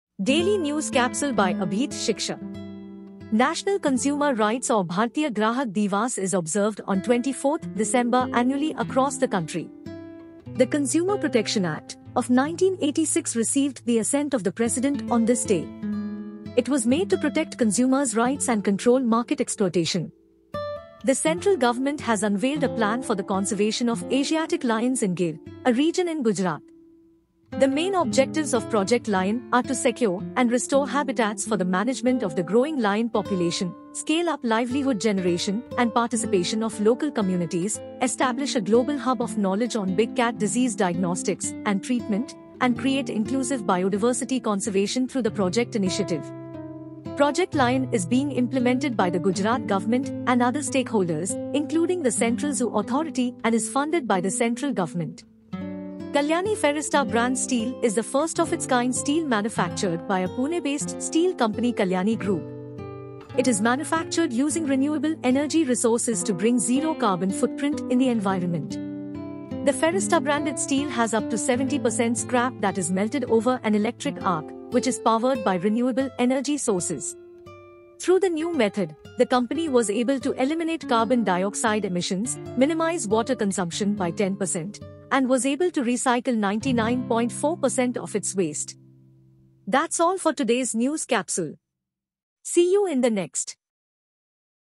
DNC 47 | Daily Analysis | The Hindu, The Assam Tribune, PIB For UPSC, APSC & other state exams
Daily update on current affairs for various competitive exams arranged in a systematic method to ensure easy grasping and cognitive intuition to better the preparation of aspirants.

24th December 2022:
1. National Consumers' Day
2. Lion@47: Vision for Amrutkal
3. Kalyani Ferresta, India's first green steel